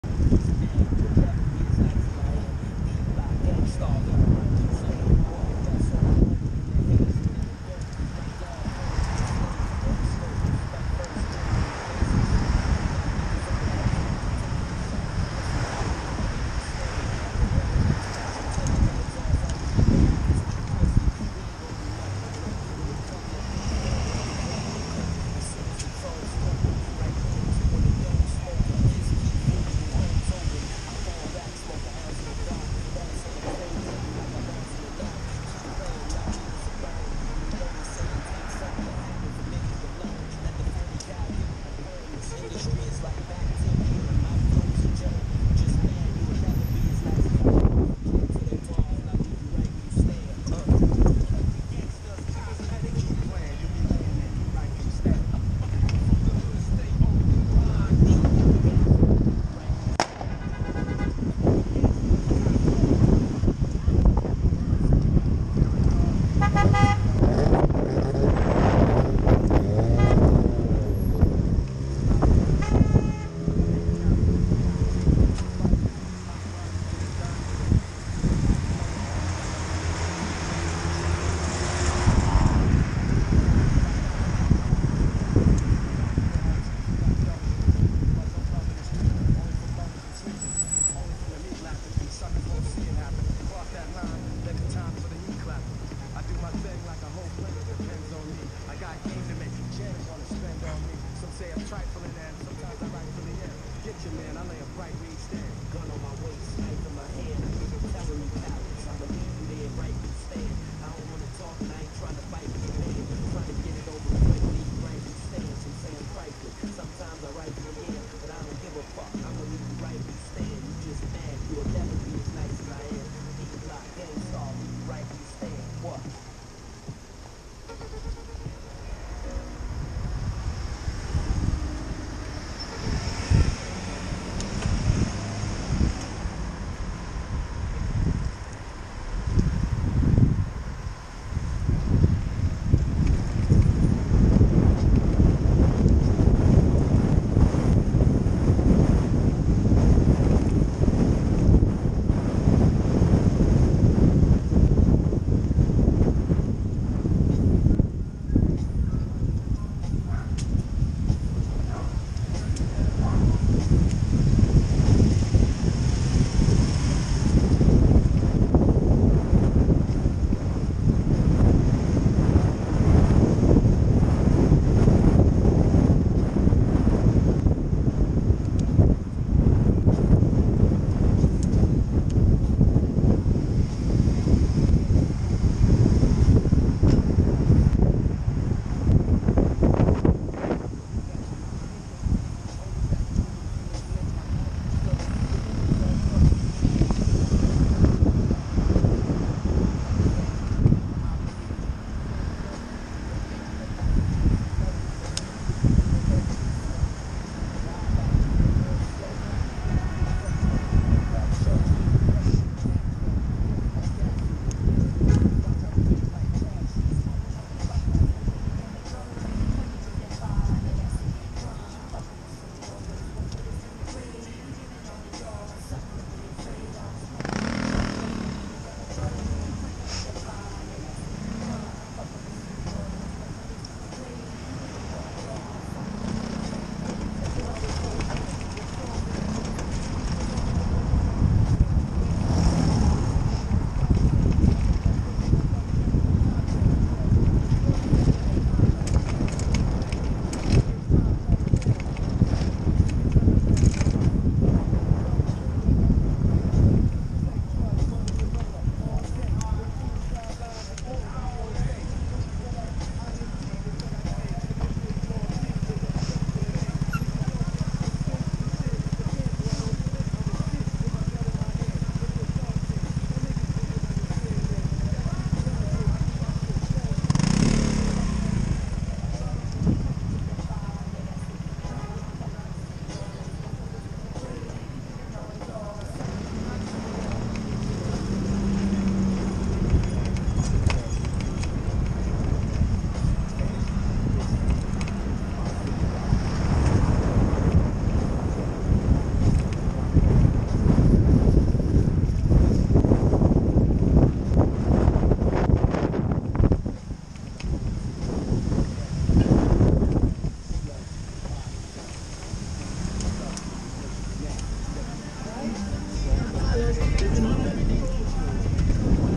Rutland Rd at night, Brooklyn - Biking NYC pov
Finally, the winter weather decided to show up, mid-November; fashionably late. Speaking of fashion, it's time to bring the wool and fleece out!

The Benevolent Berger, bicycle commute